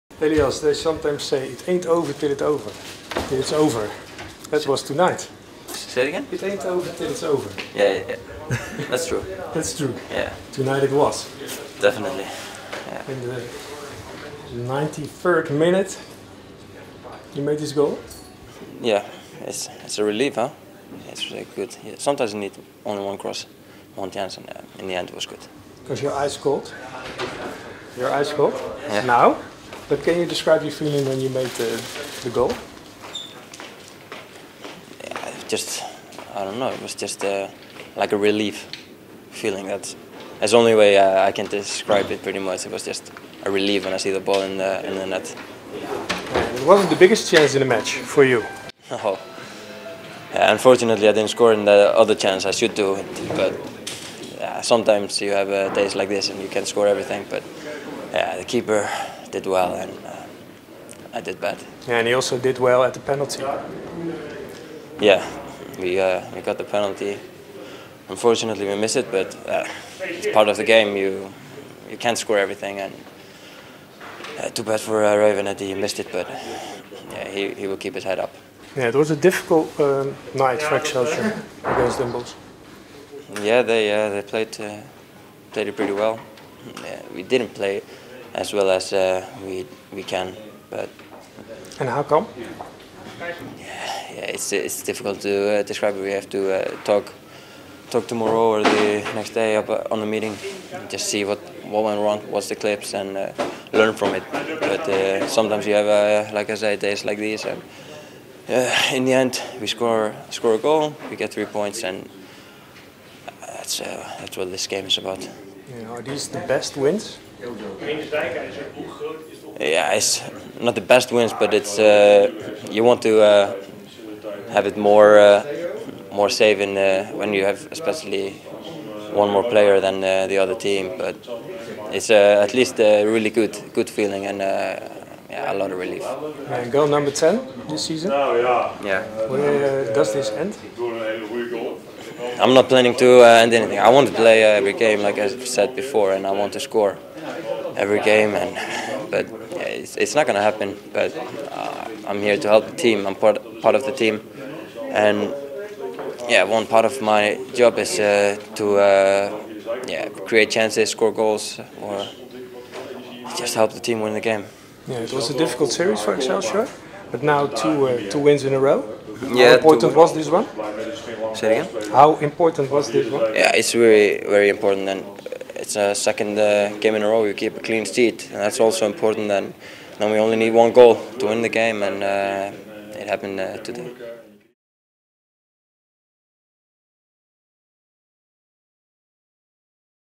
🎙️ Elías Már Ómarsson na afloop DBOEXC | "𝘿𝙀𝙕𝙀 𝙊𝙑𝙀𝙍𝙒𝙄𝙉𝙉𝙄𝙉𝙂 𝙒𝘼𝙎 𝙀𝘾𝙃𝙏 𝙃𝙀𝙀𝙇 𝘽𝙀𝙇𝘼𝙉𝙂𝙍𝙄𝙅𝙆"
Na afloop van de winst partij in Den Bosch stond Man of The Match Elías Már Ómarsson ons te woord. De ijskoude IJslander was blij dat zijn doelpunt de zege bezorgde, maar besefte dat Excelsior het duel al eerder over de streep had moeten trekken.